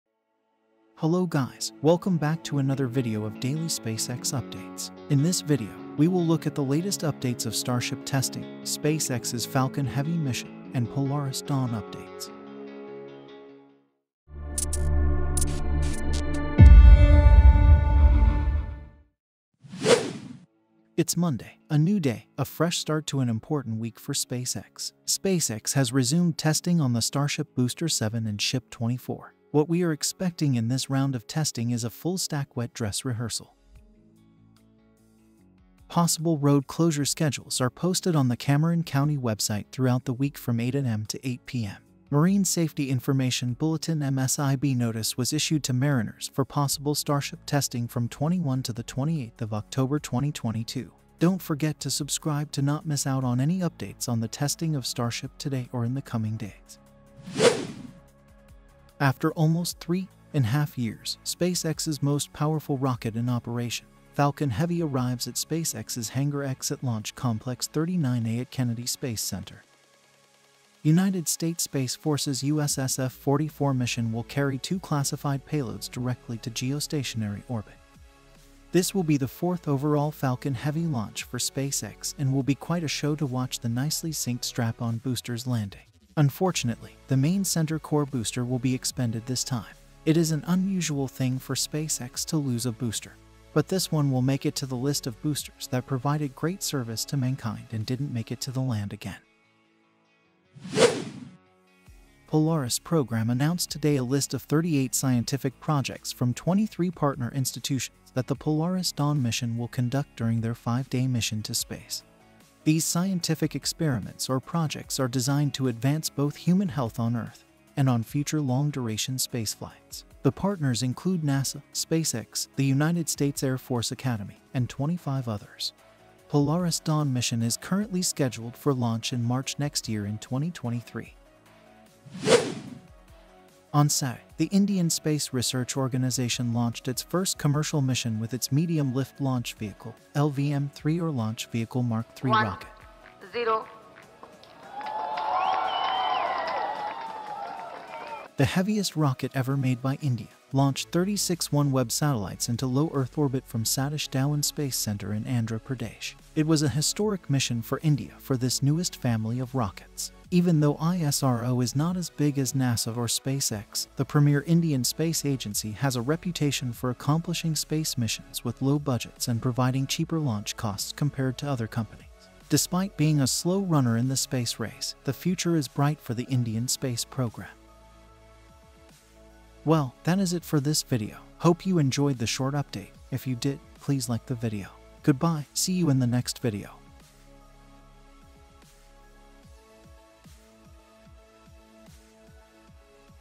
First Falcon Heavy Launch in 1230 Days + Starship Testing Resumes at Starbase | SpaceX Updates
In this video, I will be talking about the upcoming Falcon Heavy launch, the Latest updates from Starbase, starship testing, and other space stuff.